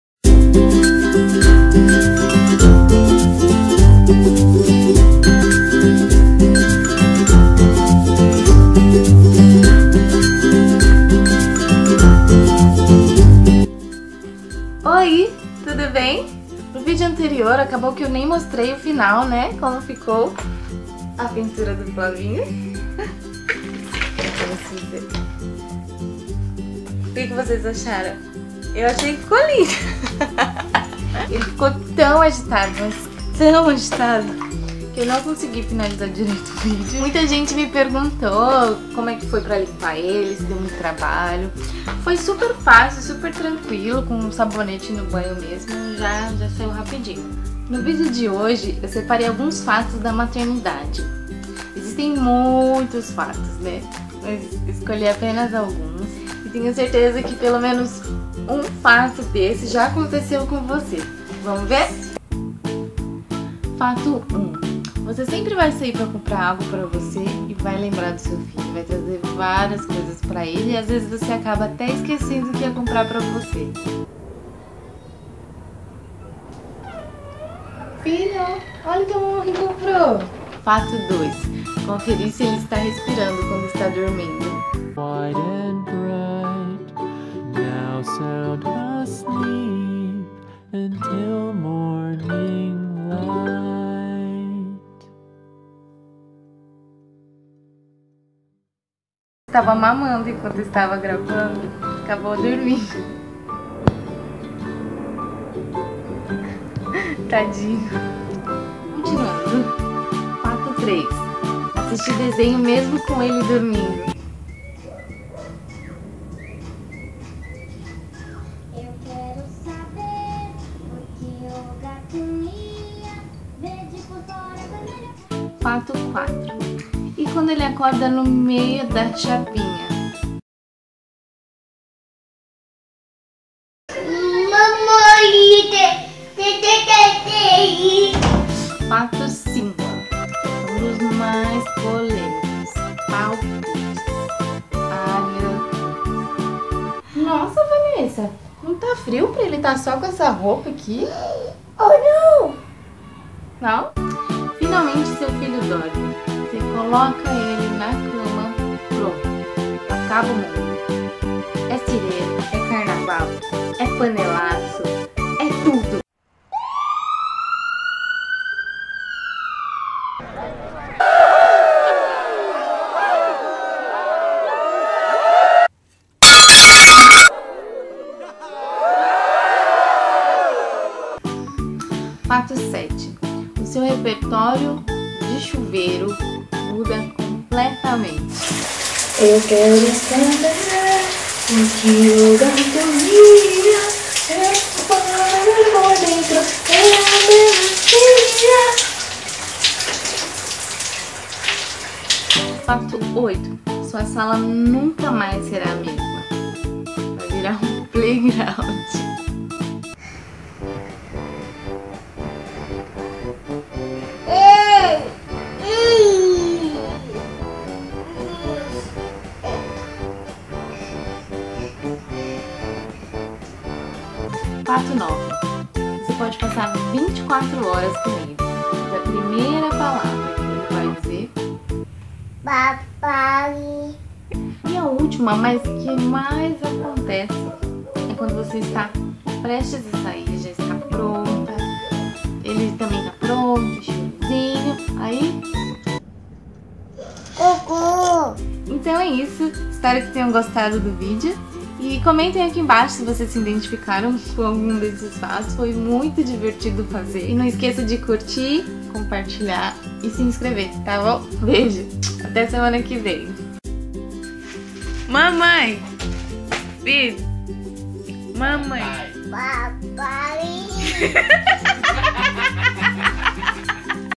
10 Fatos da Maternidade - ARTE DE SER MÃE
Oiii tudo bem?! No vídeo de hoje vou mostrar como ficou a arte que o Flavinho fez no vídeo anterior Bebê X Tinta e vou mostrar 10 fatos da maternidade. Foi super divertido fazer e espero que vocês se divirtam também assistindo. Comentem quais dos fatos já aconteceu com você, vou adorar saber. Não esqueça de curtir e se inscrever. Bjus até semana que vem =)